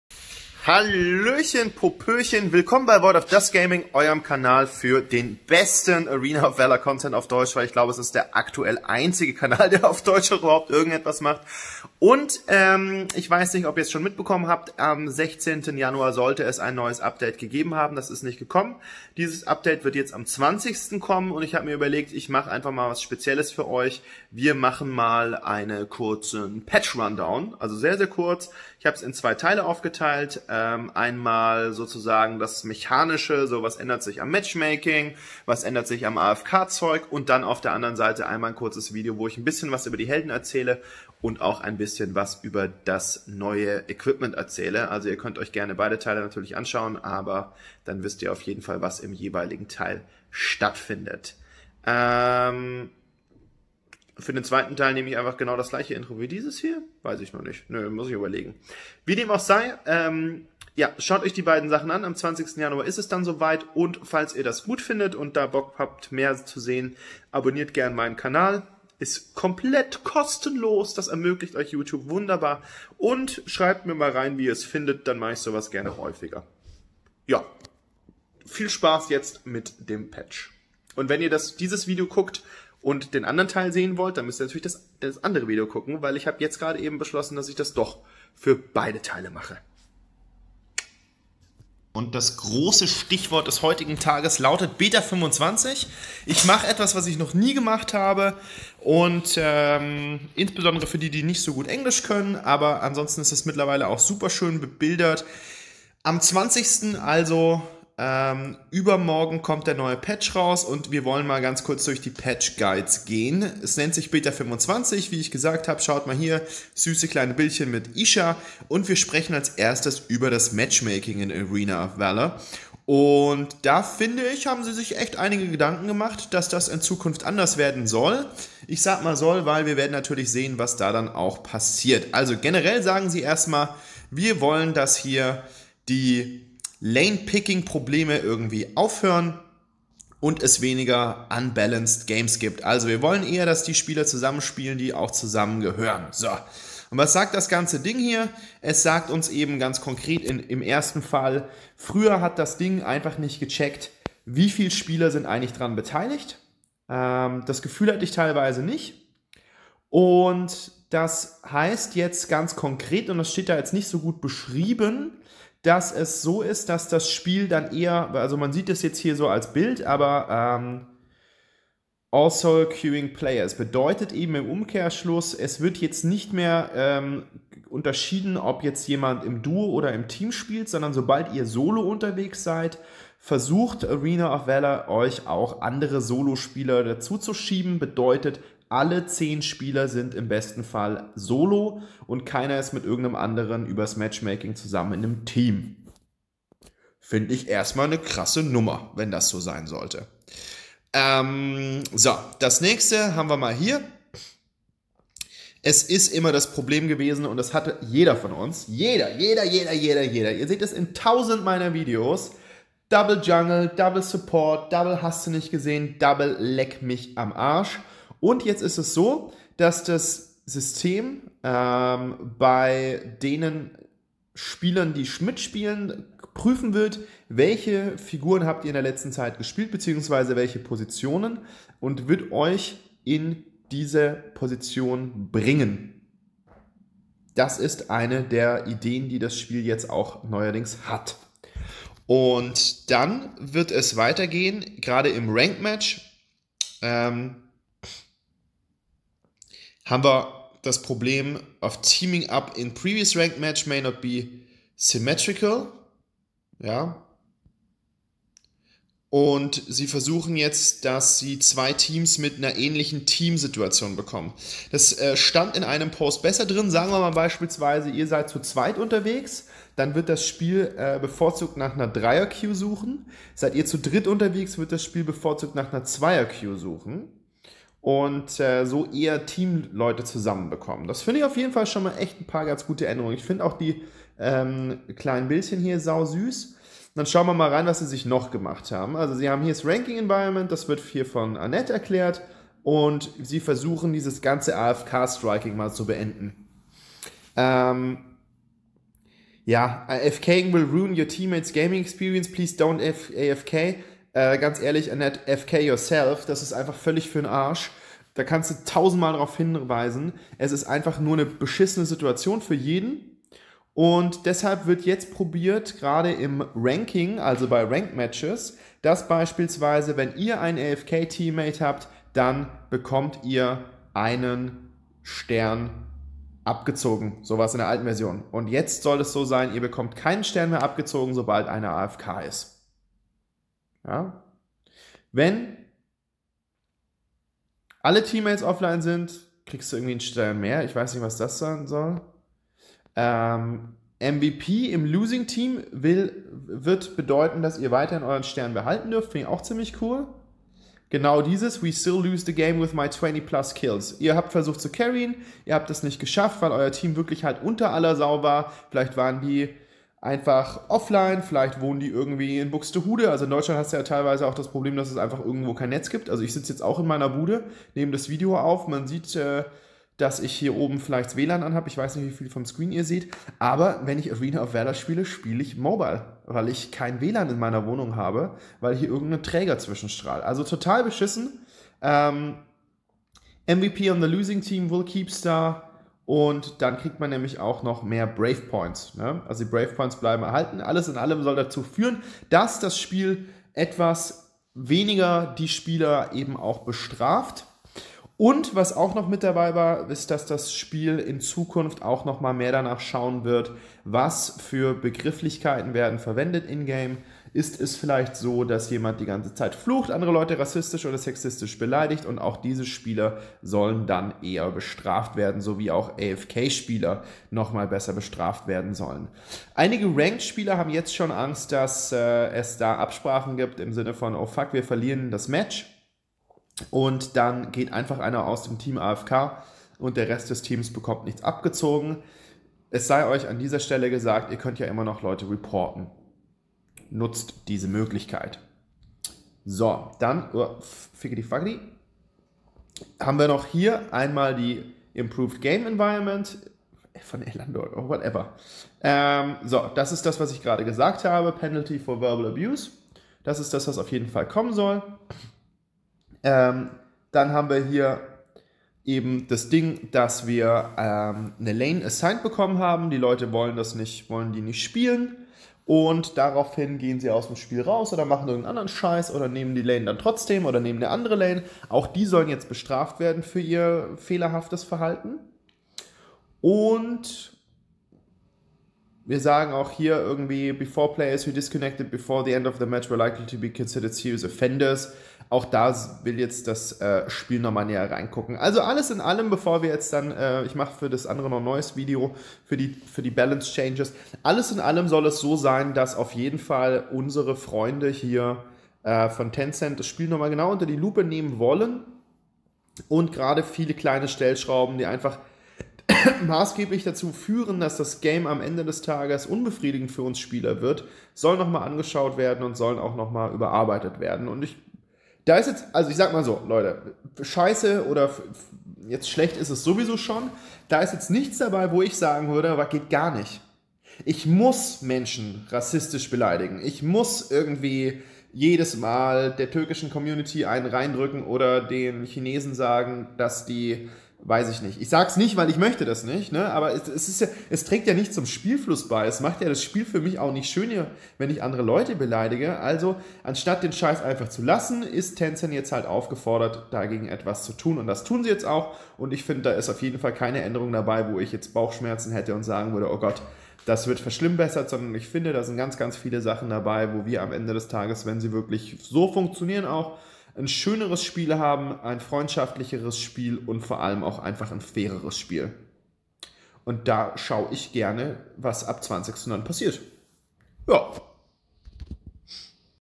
+++ [AOV Deutsch] Real Talk zum neuen Update Teil 1/2 +++ Arena of Valor / AoV / RoV
Moin Loide,

also hier DIE Absolute Weltpremiere!!! Es gibt hiermit das neue Update und meine Meinung dazu. Also was ich dazu halt so erzählen kann. Freut euch, Teil 2 kommt auch gleich.